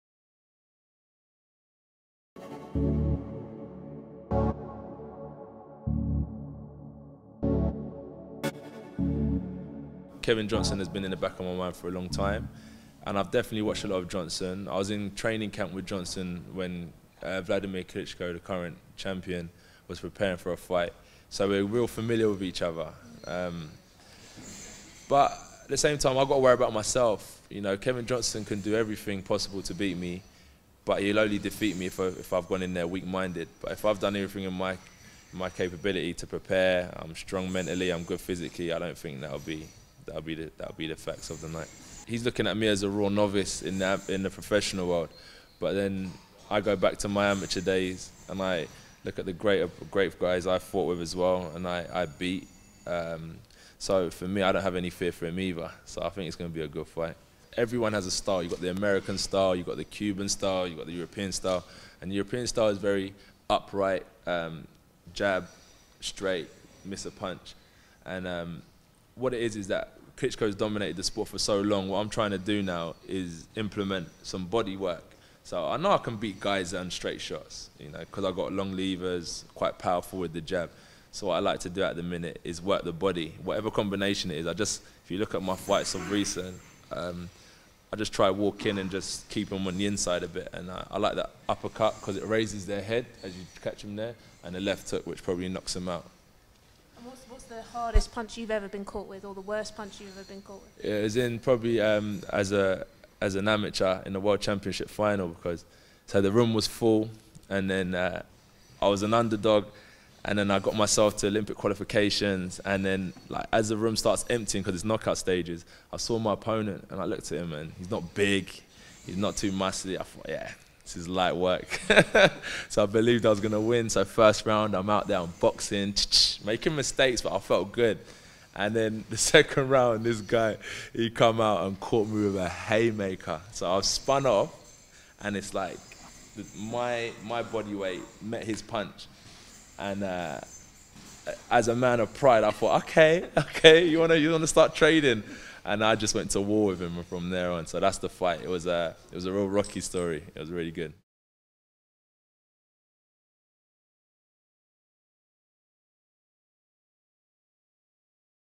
Interview with Anthony Joshua - Round 2
Anthony Joshua MBE; Olympic Gold medal winner for Super Heavyweight category interviewed by reporter from @Sportmaguk at Press Conference to announce partnership between AJ and Clinova.